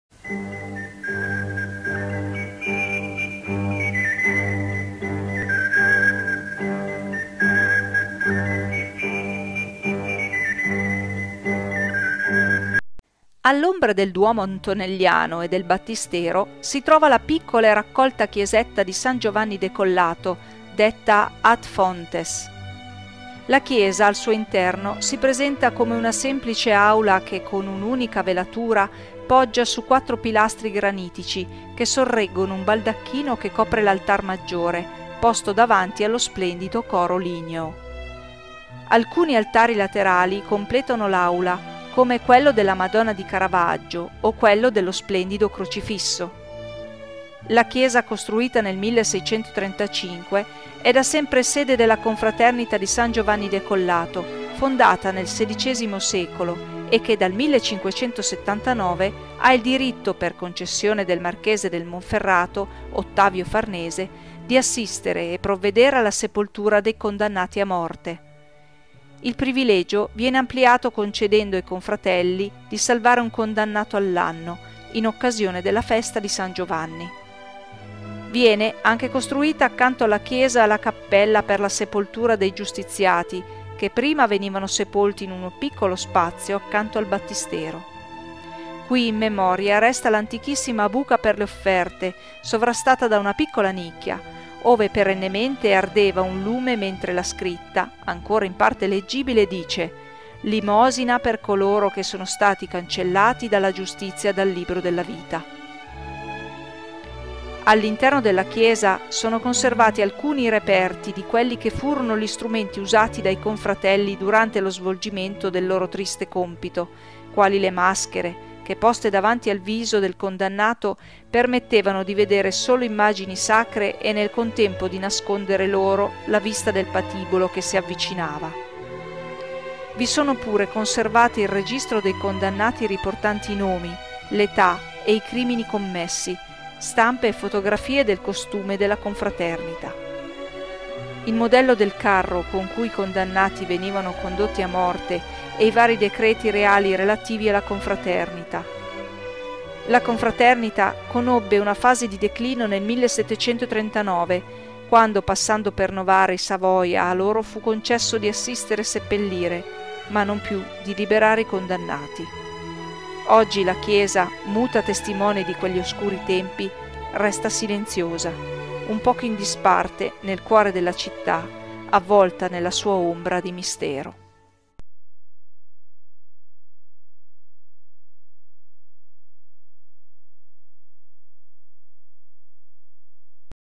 Pagina Facebook "Raccontiamo Novara": "Angolini" della città
Un modo nuovo per conoscere Novara. Un gruppo di amici che vi invitano attraverso la pagina "Raccontiamo Novara" alla scoperta della storia, tradizioni e leggende della nostra città e del contado in maniera molto semplice come lo erano i racconti dei nostri nonni nelle lunghe veglie invernali.